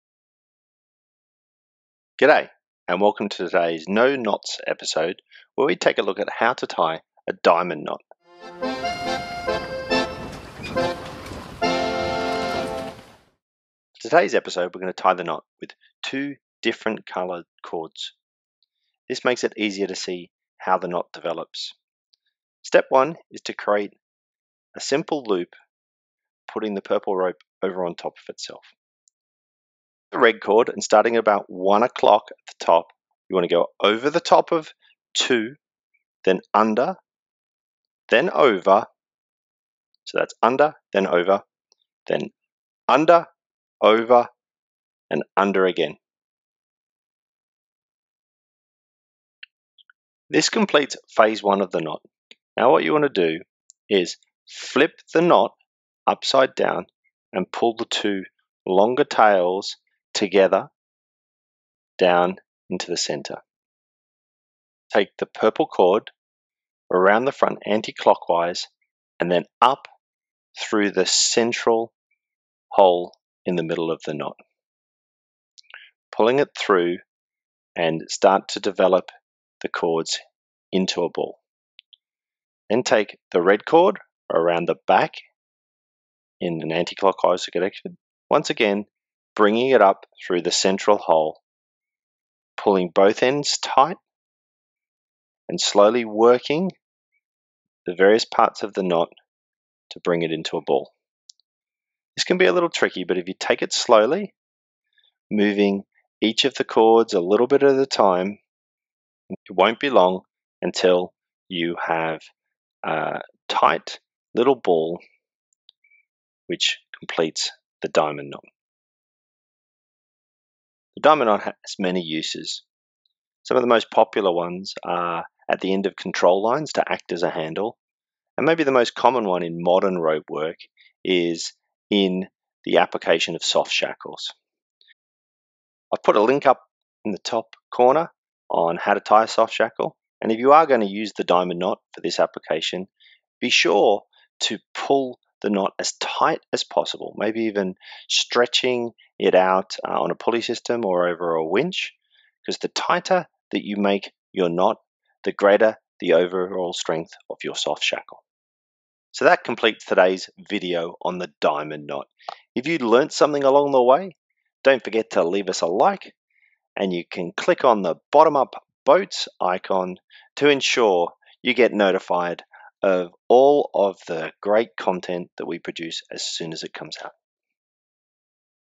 How to tie a Diamond Knot
Learn how to tie the Diamond Knot with two strands of rope in less than 3 minutes. The Diamond Knot creates a ball that can be used at the end of ropes as a handle or stopper, more recently the Diamond Knot has been used in the creation of soft shackles.

The Diamond Knot is easy to tie and uses minimal rope once you have learnt how to do it. We use two different colours of rope to help you see how the knot is constructed.

If you are going to use the diamond knot in your soft shackles you should tension the knot into the tightest ball possible. This will increase the overall strength of your soft shackle.

The diamond knot can be tied with varying strands of ropes be sure to subscribe to our channel so you are notified of all the new content created by Bottom up Boats.

Now all you need to do is hit the play button to learn how to tie a diamond knot.